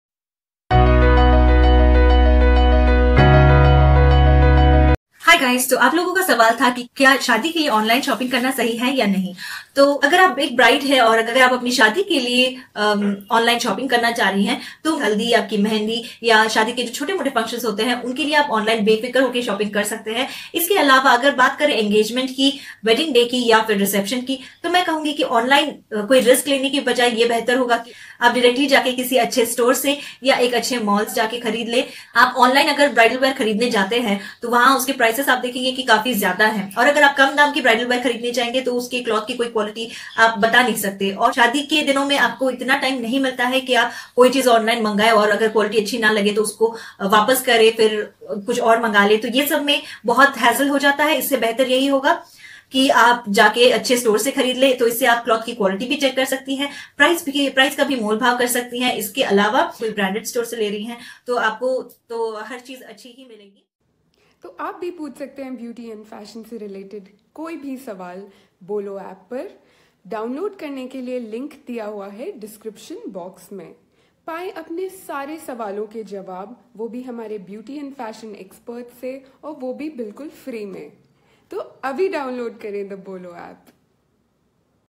kya shaadi ke liye online shopping karnaa sahi he? - by Poornima - Bolo App
1,00,000+ se bhee jayda answers de chuke hain expert abhi tak Bolo App par, aap bhee paayein apne questions ke answers

Poornima's answer to क्या शादी के लिए ऑनलाइन शॉपिंग करना सही है?
Poornima's answer to kya shaadi ke liye online shopping karnaa sahi he?
Poornima's answer to Is it fair to shop for wedding online?